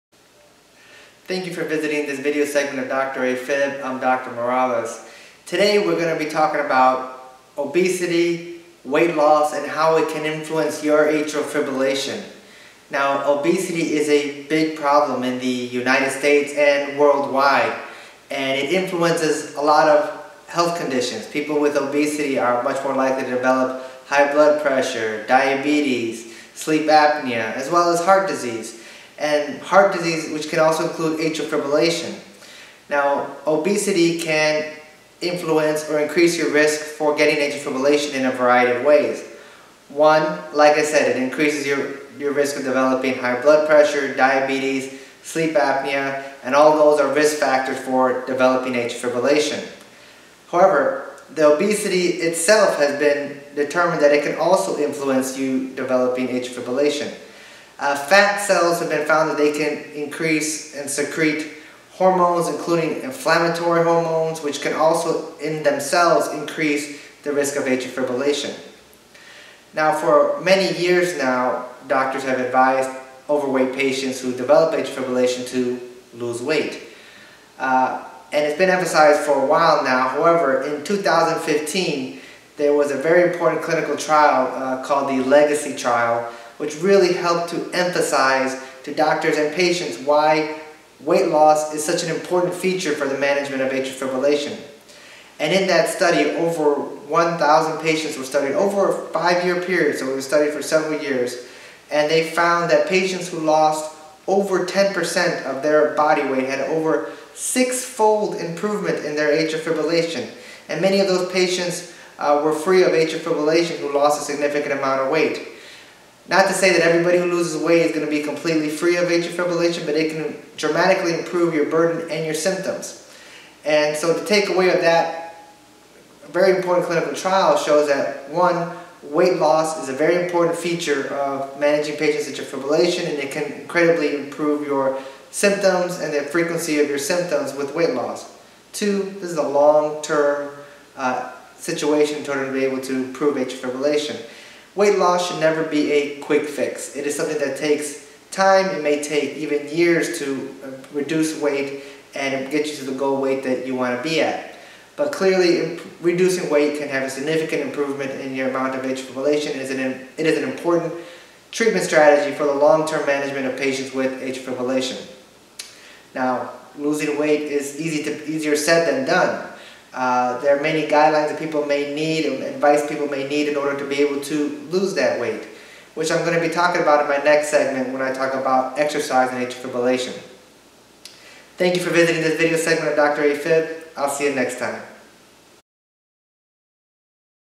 How Can Weight Loss Improve Atrial Fibrillation?
In this video I discuss how weight loss can improve atrial fibrillation.

Information is strictly educational.

Check out the KardiaMobile and KardiaBand - Mobile ECG for at home monitoring of atrial fibrillation